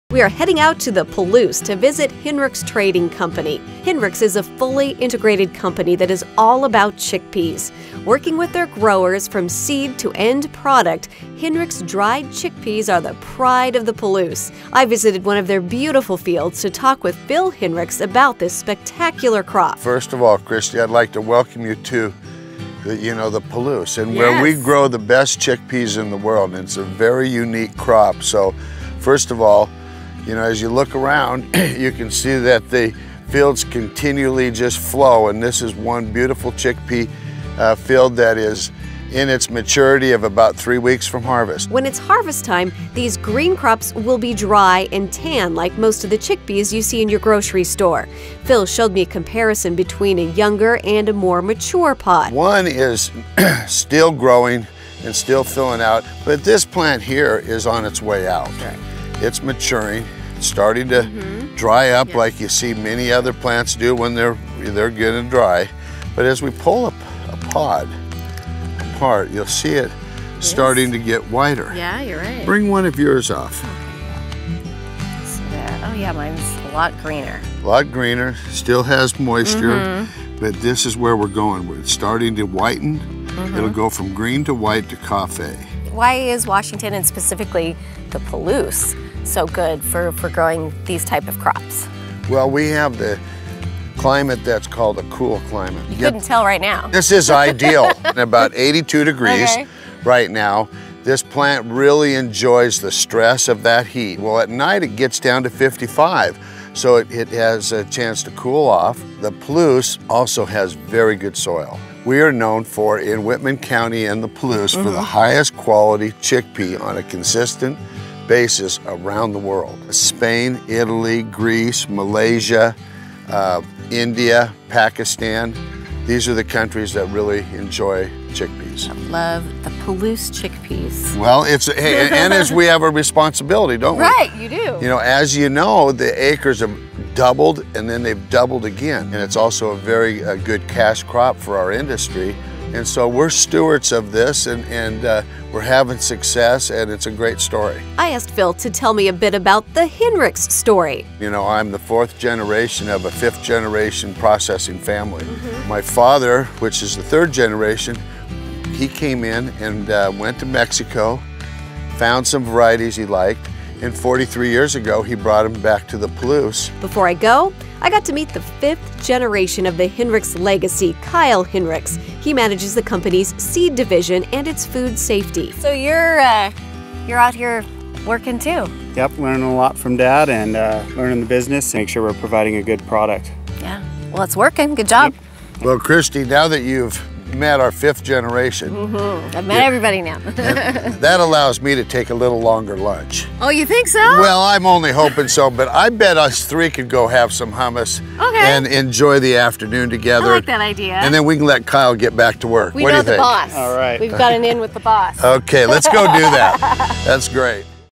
Growing Chickpeas | Hinrichs Trading Co. | Washington Grown
Visit Hinrichs Trading Company with us as we visit the Palouse and see how the grow chickpeas aka garbanzo beans!